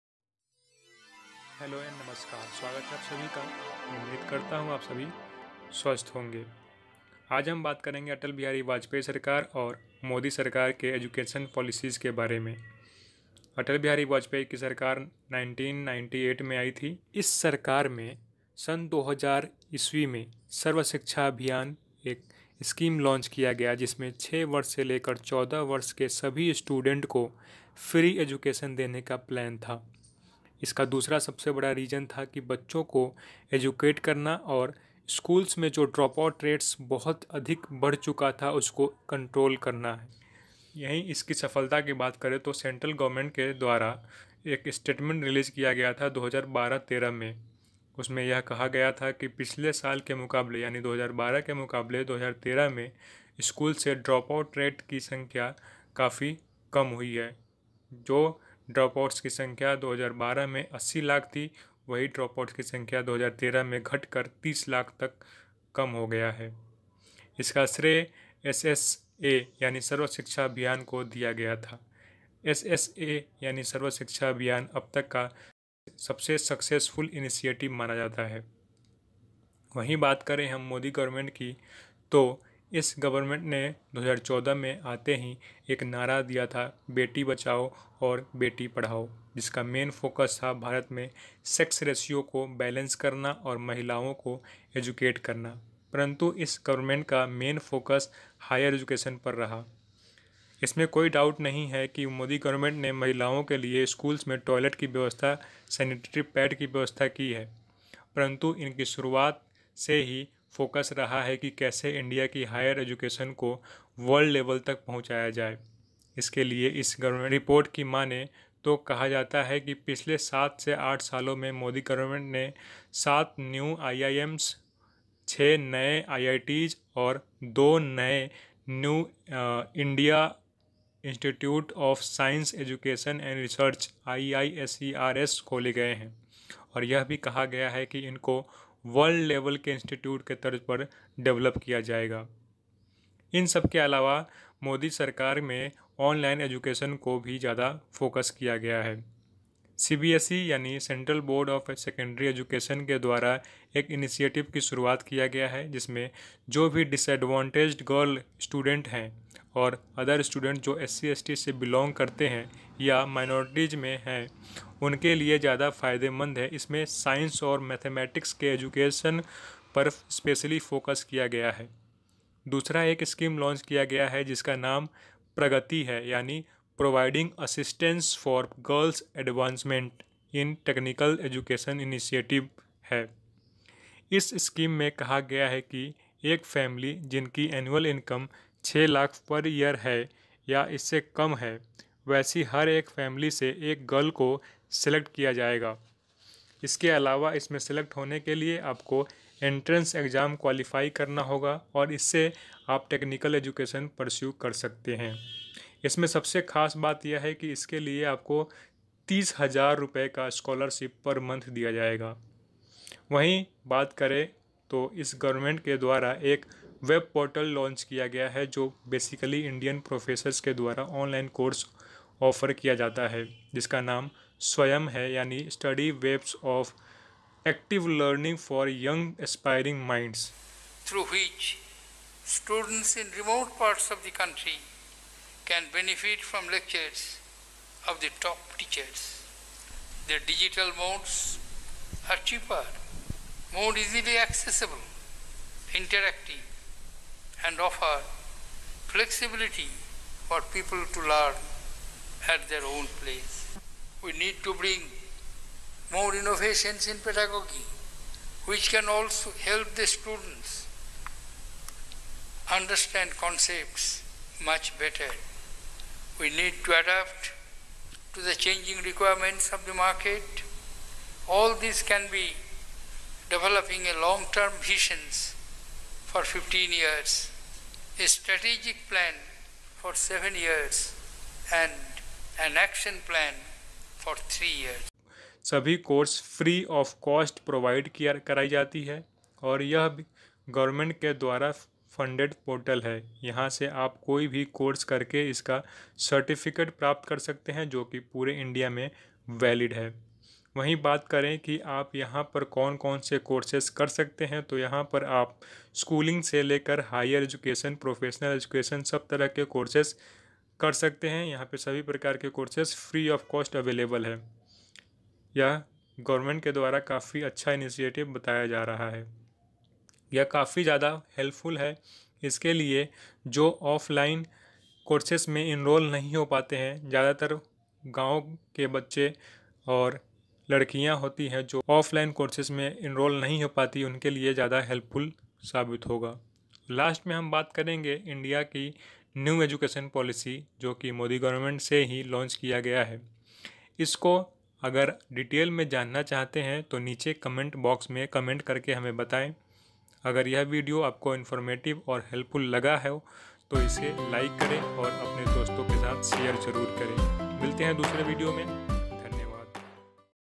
WHO IS THE BEST PM FOR INDIAN EDUCATION REFORMS / ATAL OR MODI ?
Who is the best pm of india for indian education reforms
Atal govt. Or modi govt.
Thankyou keep watching my videos
Umesh kumar.